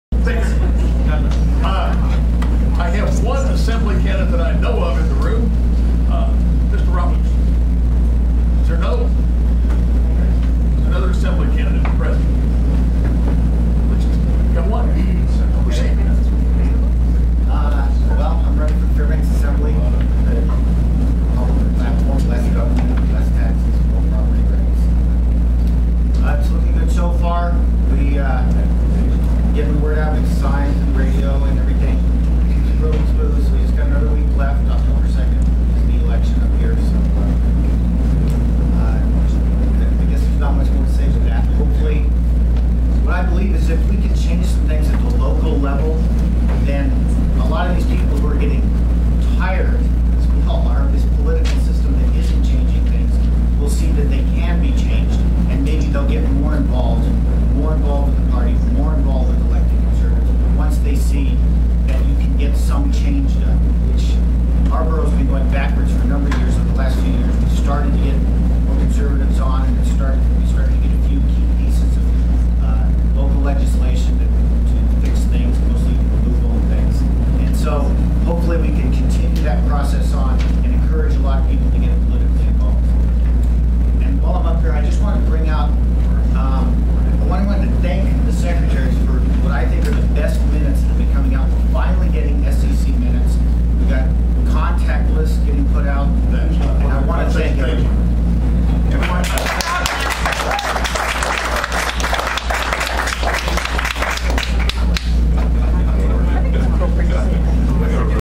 Randy Ruedrich unfairly cuts short a person running for assemblyman speaking time.
Randy Ruedrich unfairly cuts short assemblyman Lance Roberts speaking time when wish to thank the secretaries for doing a decent job of getting minutes out.  He was supposed to have three minutes to speak but Randy Ruedrich not liking where the speech was going stopped him from finishing his presentation.